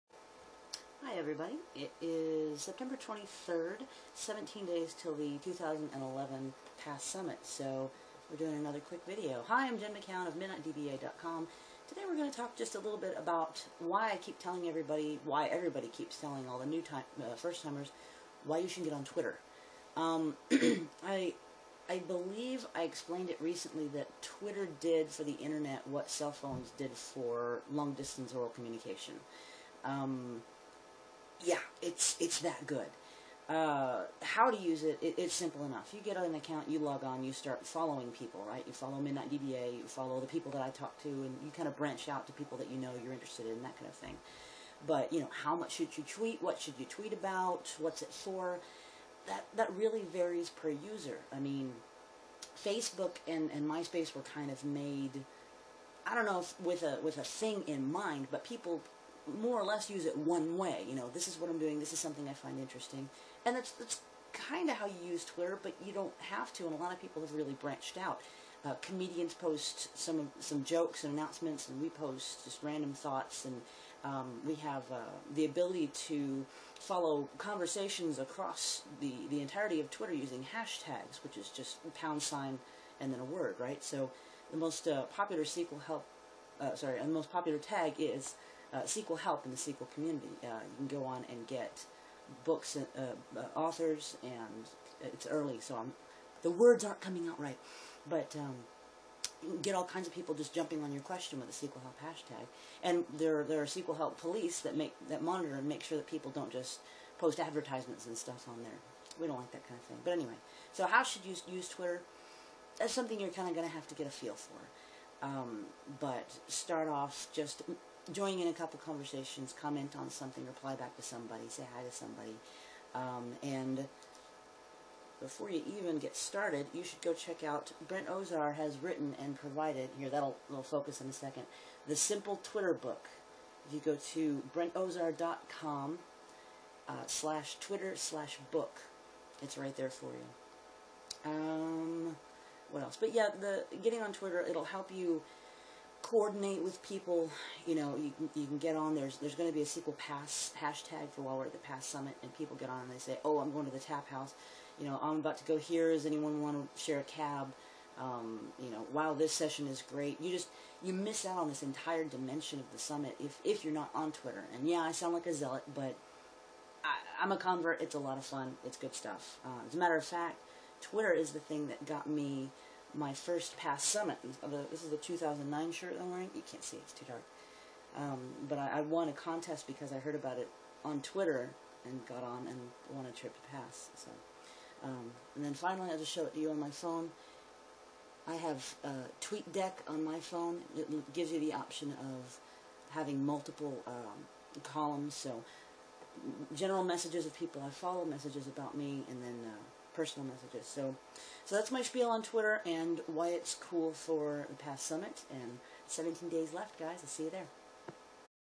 PASS Summit 2011: T-17 Days ((Twitter!)
I really REEEEALLY like the PASS Summit in Seattle. There's a lot going on with us and with PASS, so I'll post a few of these little vids in prep for the big event.

In this 3.5 minute episode: Why Twitter for PASS Summit? How do people use it?  Now, go read Brent Ozar's Simple Twitter Book

Oh, and go read my blog at MidnightDBA.com/Jen, just for the sheer joy of it.